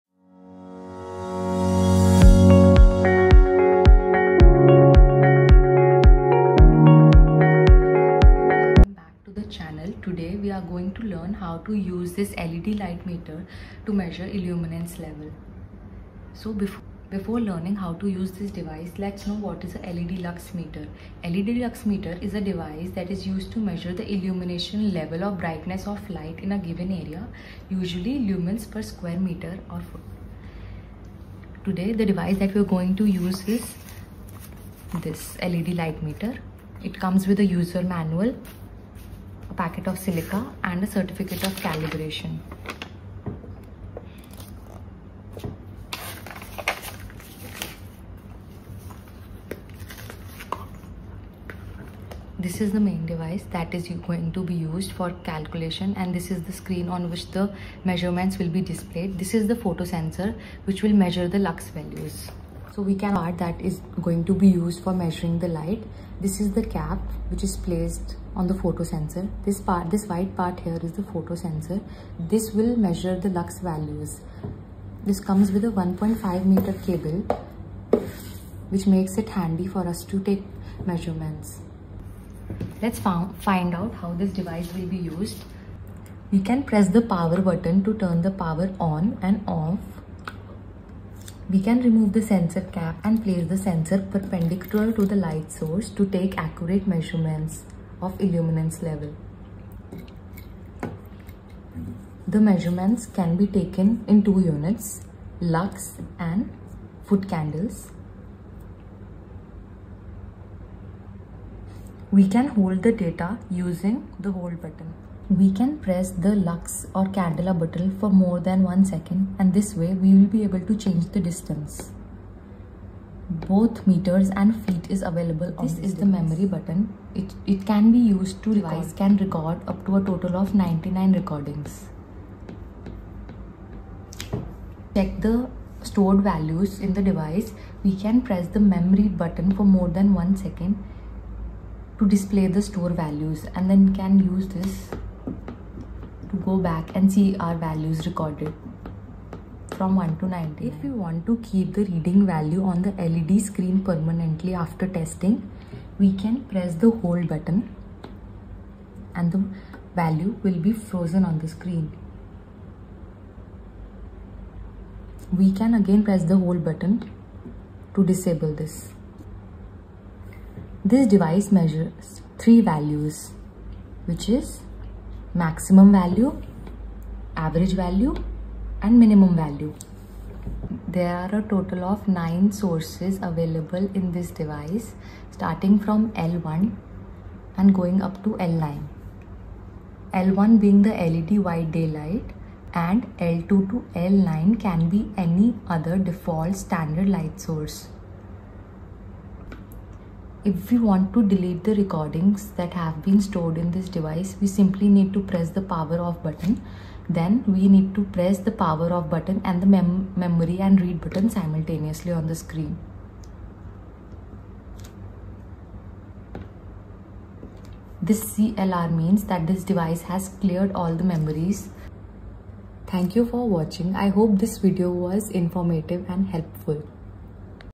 How to use LED Light Meter | Lux Meter | Accurate Light Meter | Instructional Video | Unboxing
Learn how to use the LED Light Meter step by step!